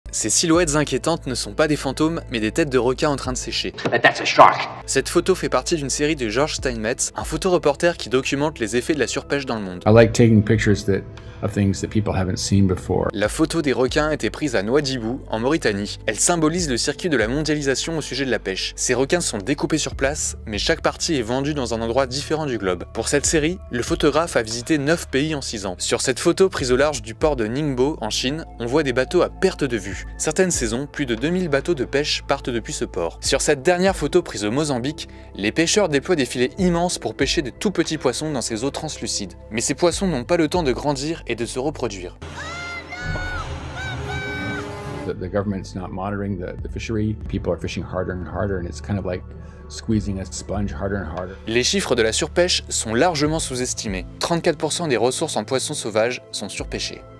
Surpêche : les images d'un désastre
Le photographe George Steinmetz parcourt les océans avec un but : montrer le mal que la surpêche fait à notre planète.